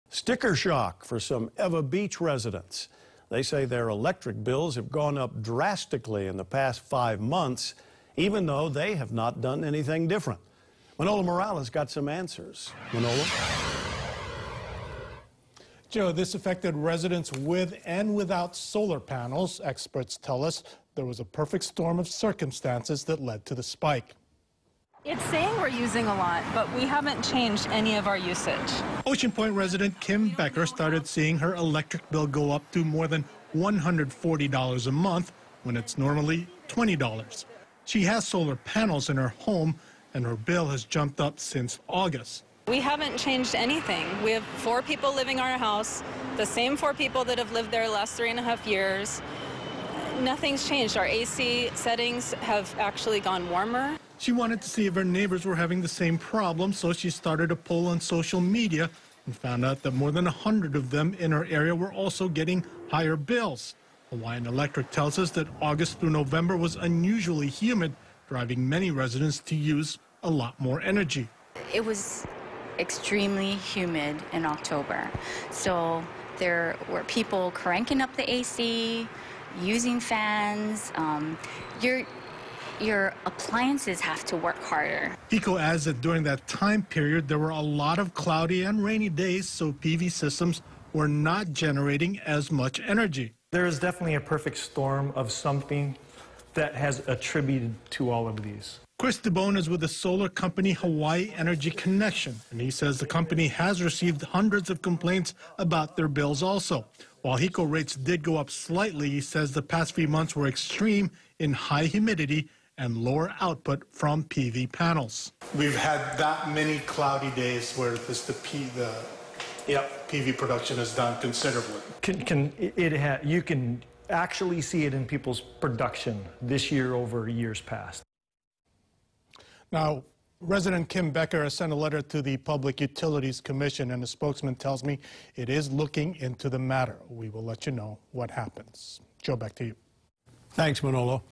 HECO bills spike over unusually humid months
Sticker shock for some Ewa Beach residents. They say their electric bills have gone up drastically in the past five months, even though they haven't done anything different.